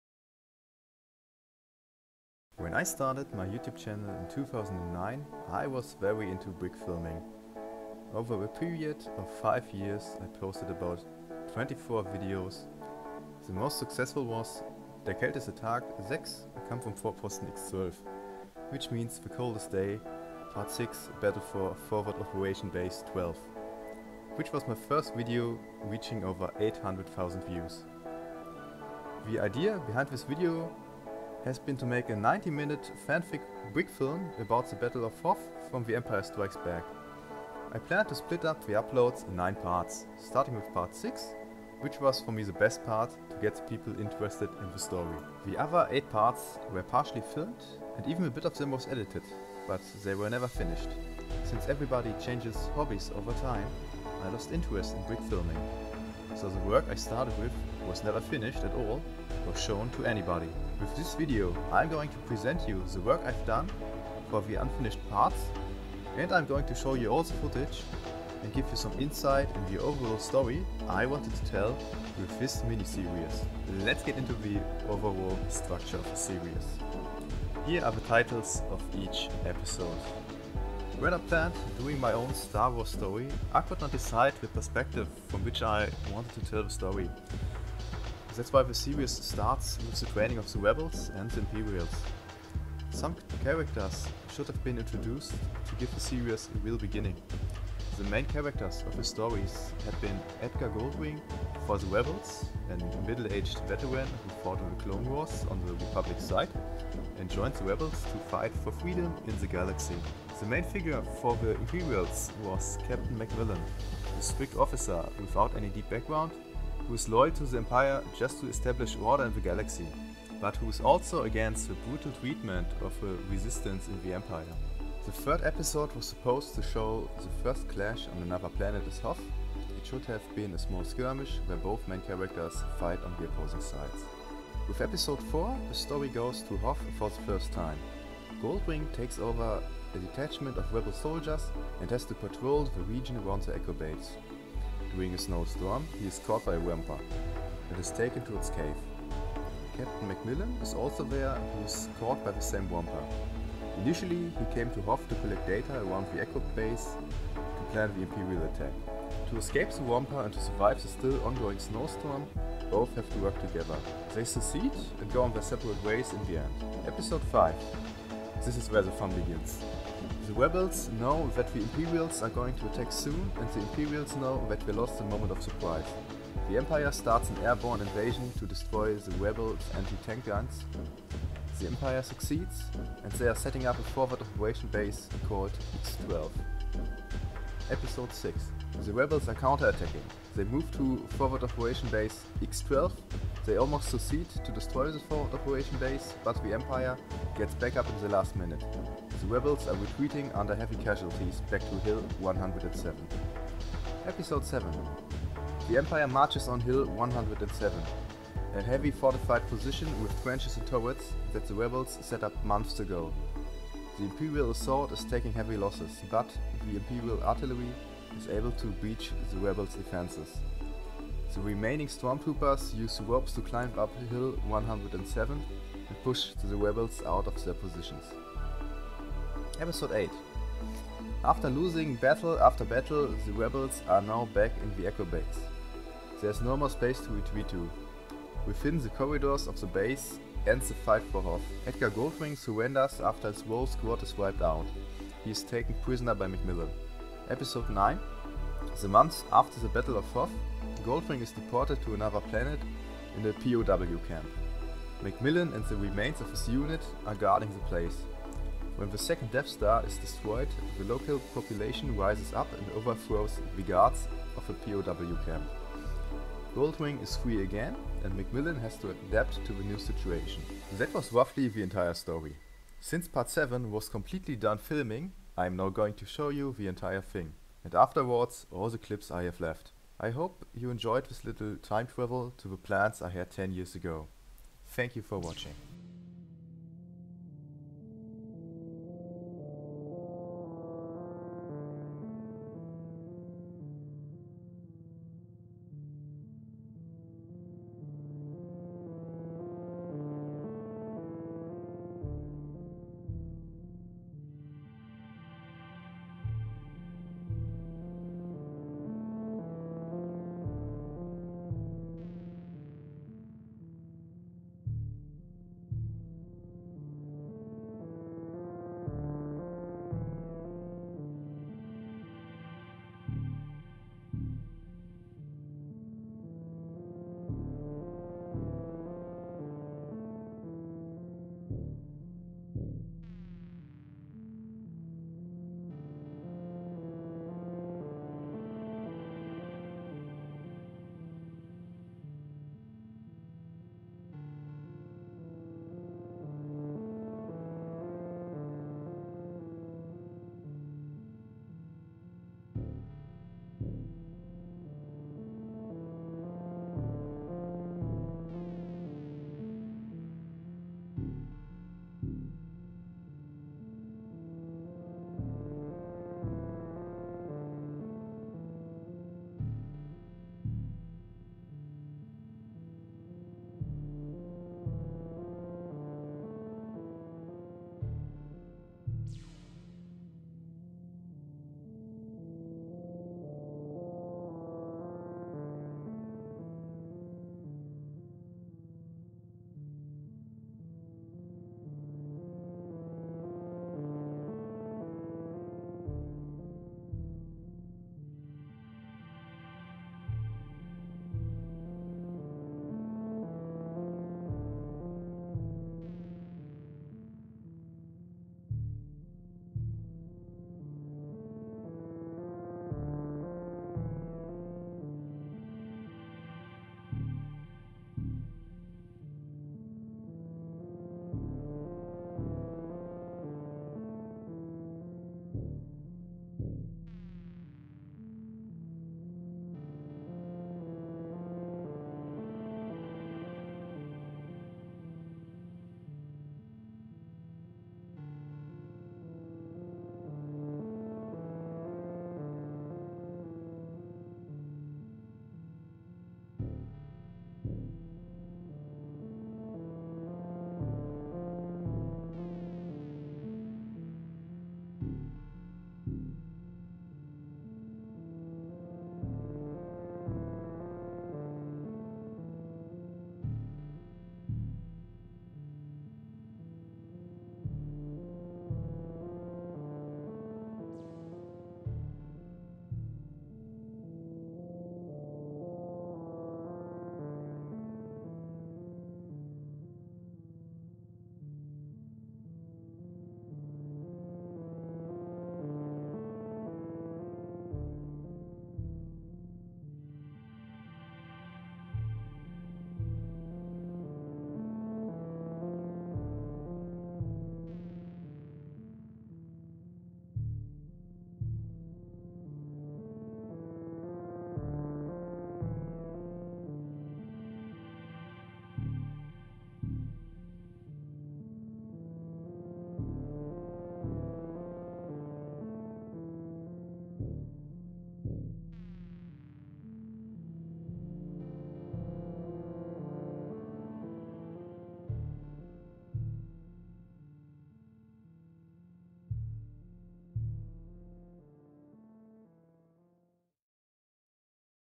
End of an ambitious brickfilm project - Unfinished brickfilm from 2011
Special thanks goes out once again to the former voice actors: Stormtrooperlego
MrLegoCloneCommander
TheStarwarsfreak15
Spidergeck
Skellhouseful
MrLPfan95
MrLSWFan
Gitarrenliebhaber

Minor Fawn
for the design of the medic kit